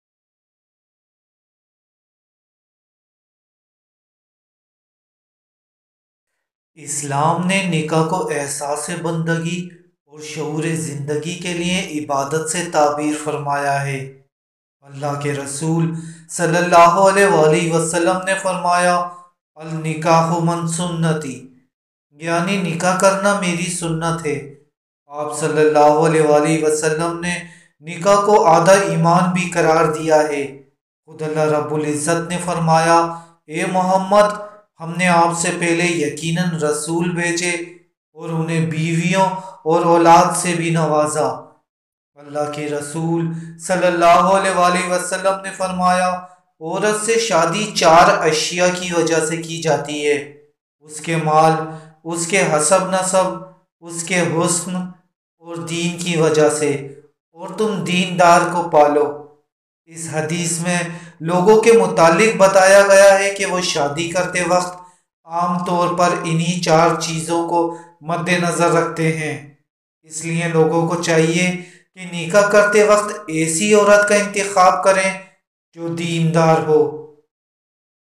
Shadi Kaisi Ladki Se Karna Chahiye l Kaisi Ladki Se Shadi Karni Chahiye lIse Ladki Se Shadi Mat Kare
As'salalm o Alekum!

About This Video👇

Viewers is video mein ap Hazrat Muhammad SAW Ka Farman Janen Gy Jis Mein Ap ne Farmaya Shadi Karte Waqat Larki Mein 4 Chezen Dekhen.

Watch Comments.

Voice, Concept, Research & Produced by:
(Aurangzeb)

Ya Qawiyyu Kamzoor Demagh ka Wazifa

Keep Supporting💞